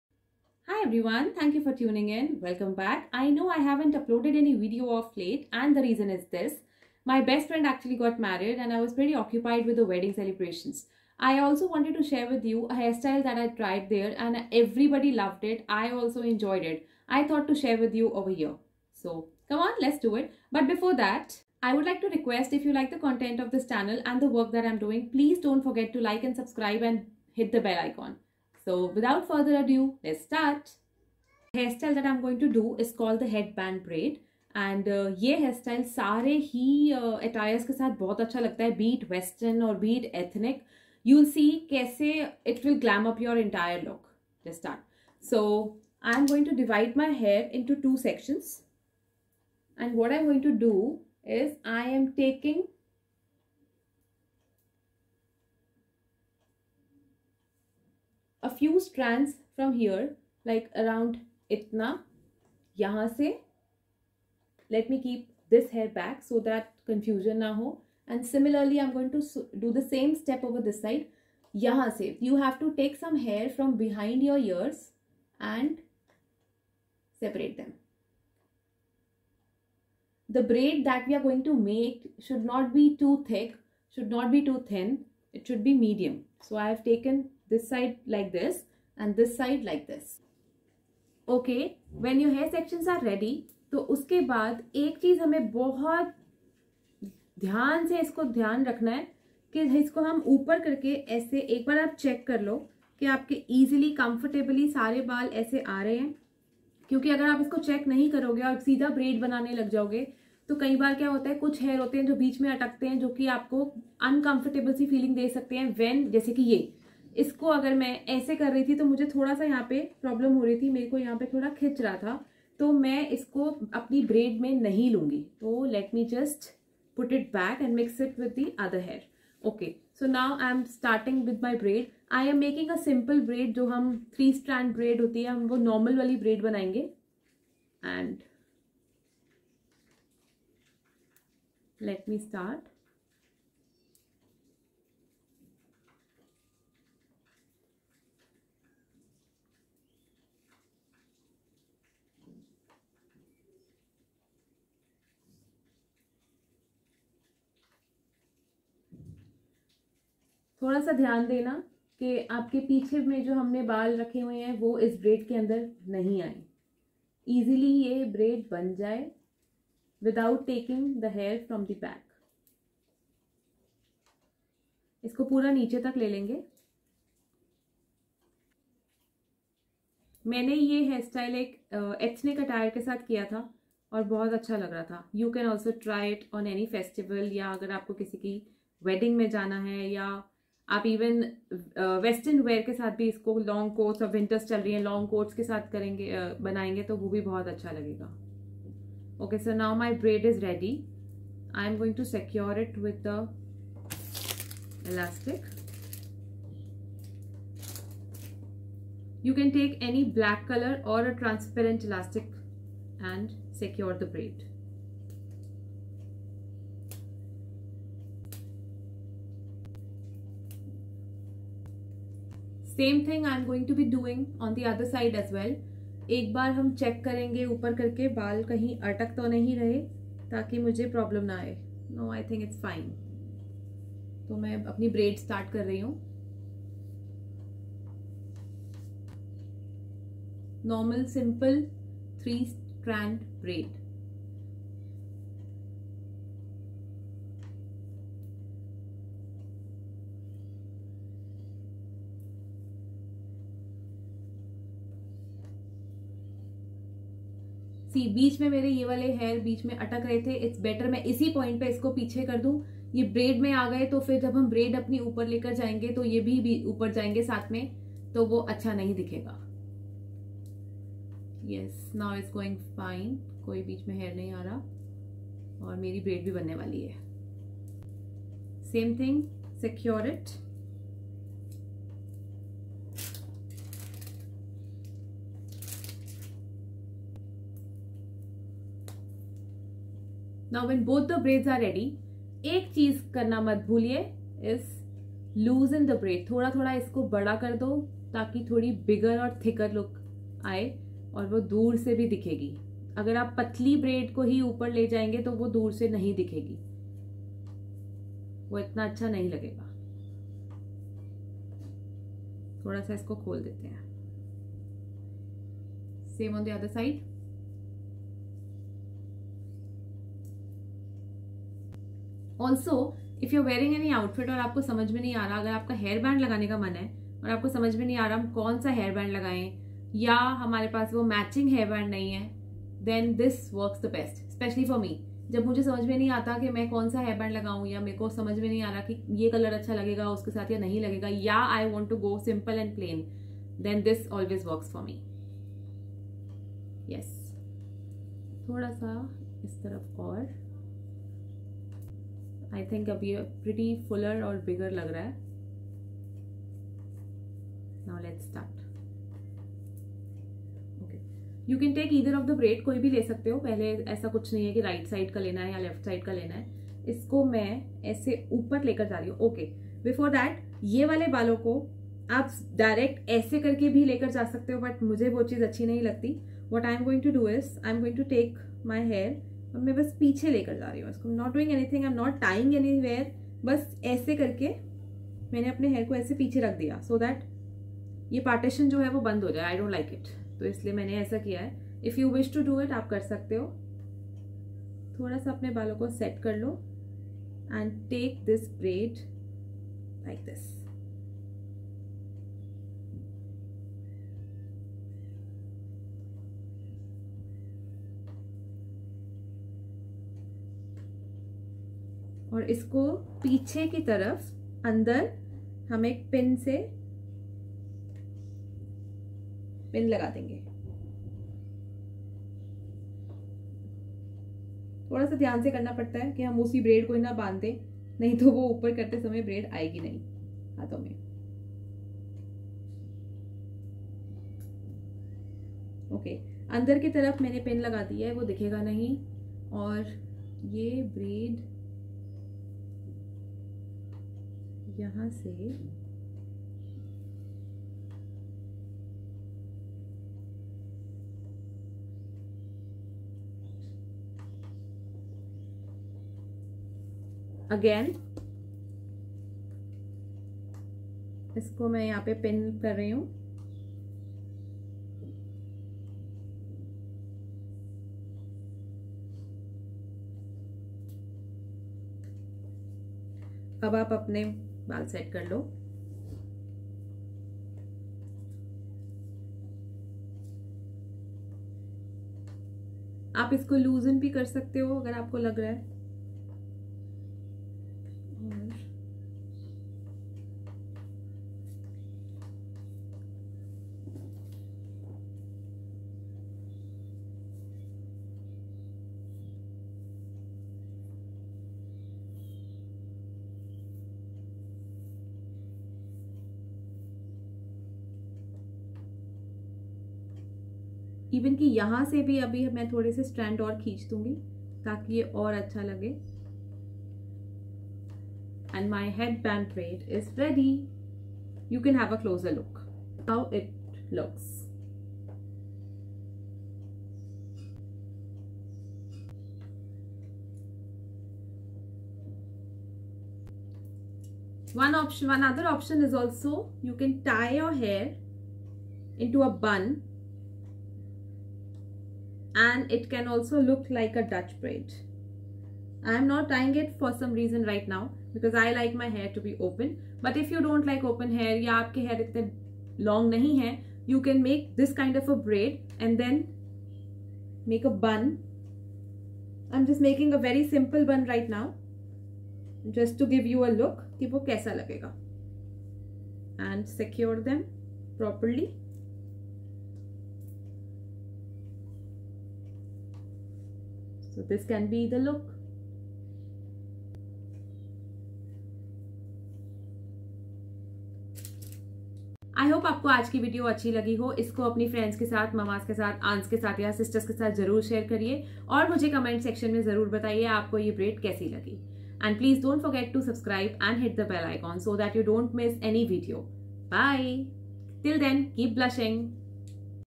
Easy Wedding Guest Headband Braid Hairstyle, Quick 5 minutes Hack
Try this Easy Wedding Guest Headband Braid hairstyle..It is super easy 5 min Braid that will Glam you up.